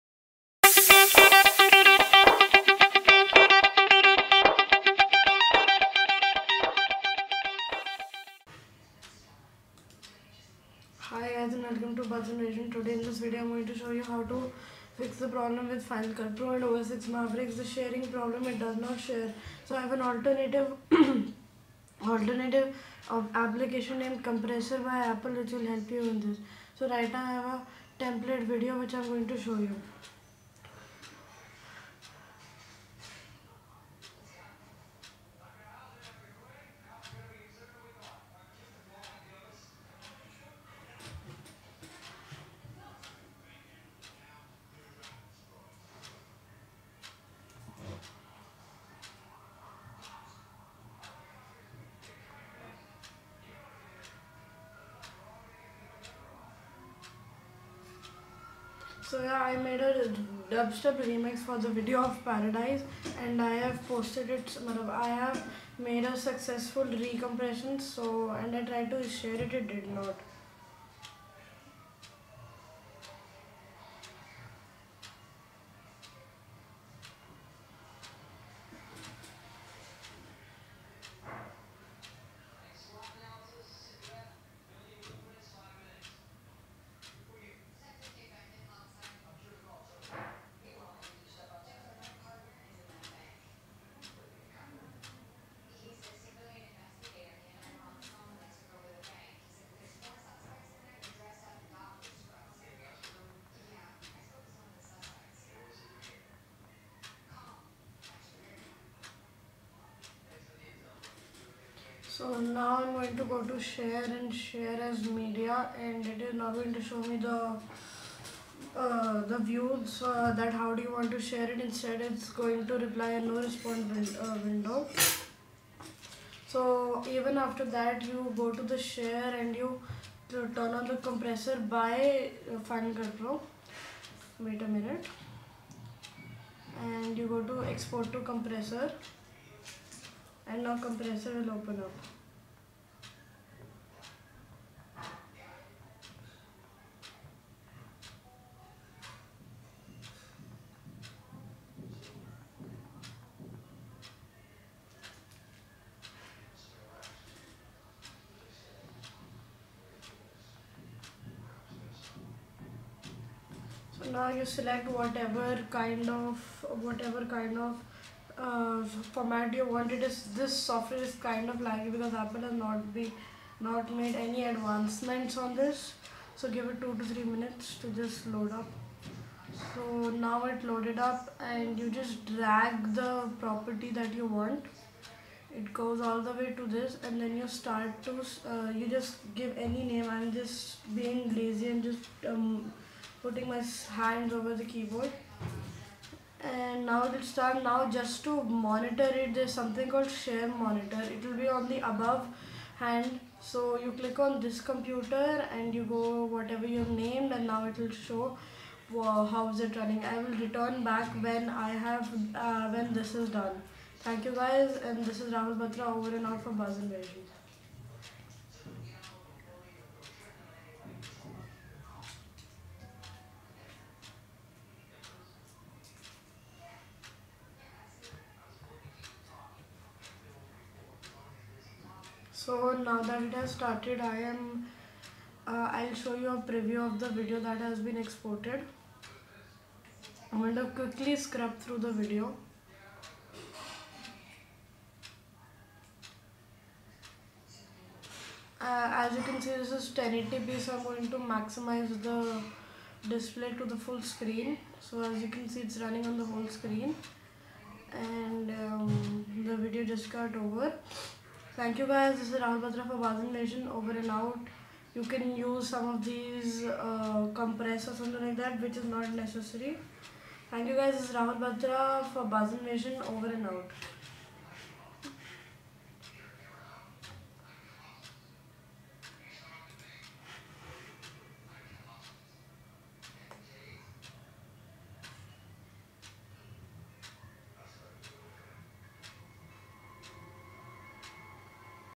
How to fix the problem of Final Cut Pro share in OSX MAVERICKS
the problem in OS X Mavericks now solved

                                Technology made simply
                                                                      -Rahul Batra